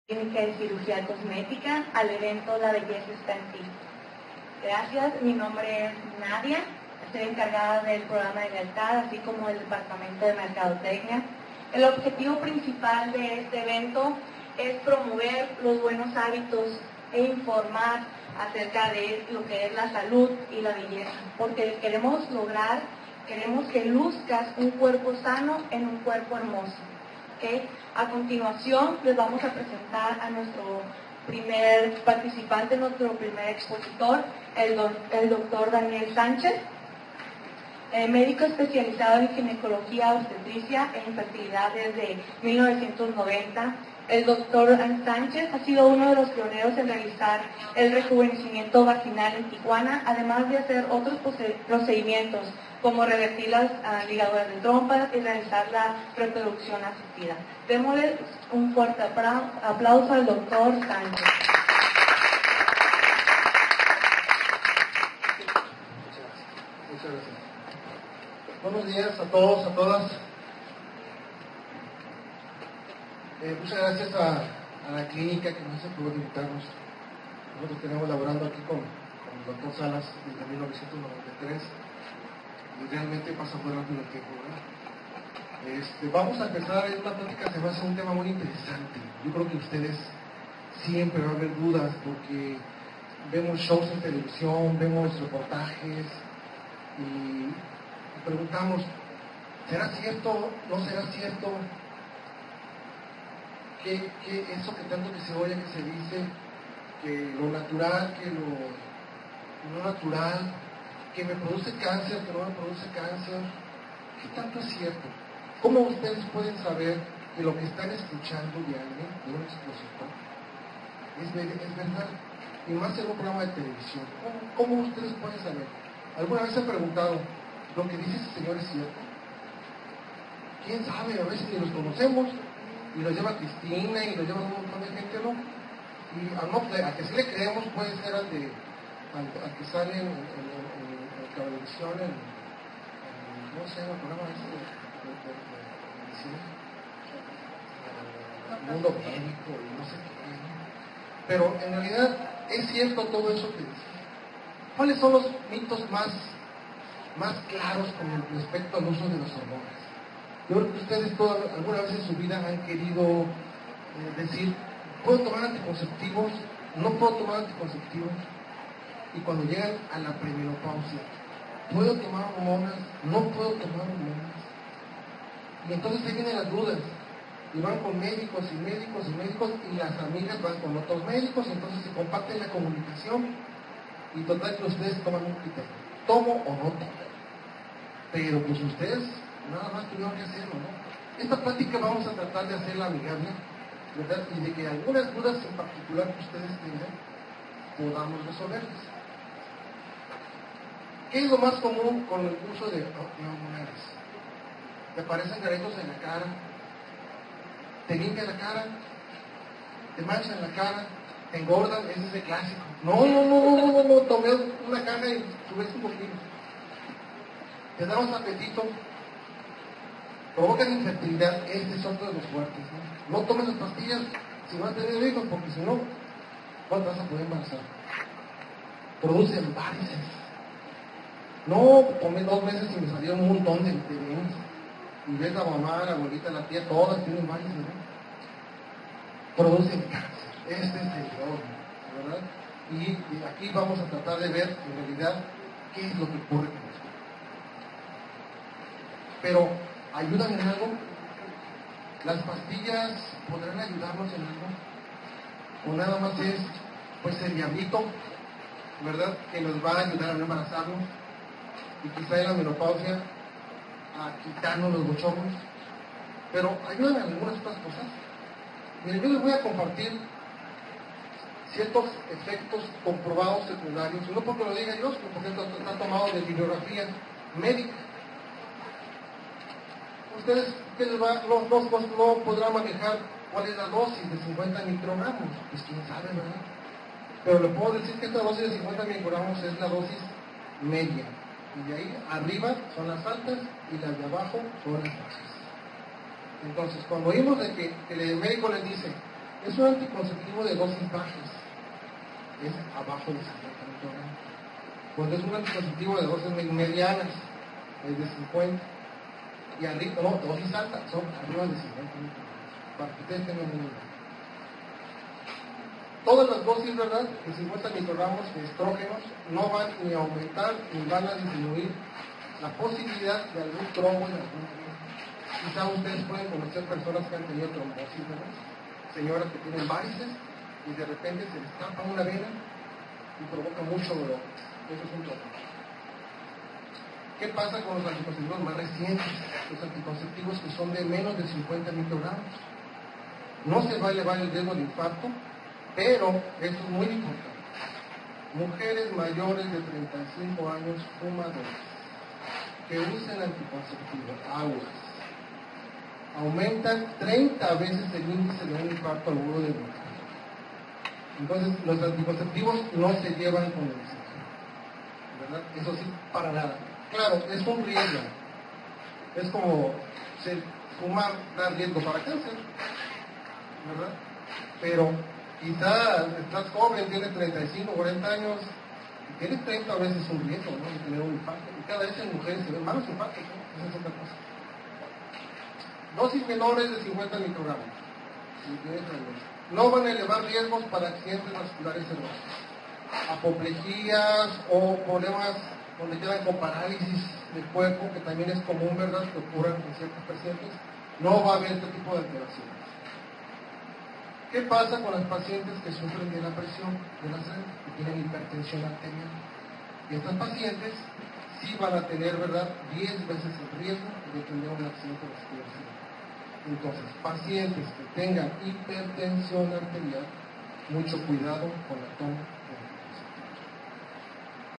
Conferencia Mitos y Realidades de Tratamientos hormonales Parte 1_CCCI
Esta conferencia fue impartida por nuestro Cirujano Especialista en Gineco-Obstetricia Dr. Daniel Sánchez Martinez, dentro del evento la belleza esta en ti, realizada para todos nuestros pacientes de Clínica de Cirugía Cosmética, como anfitrión de este evento fungió Dr. José Luis Salas Martínez, Director de Clínica de Cirugía Cosmética e Integral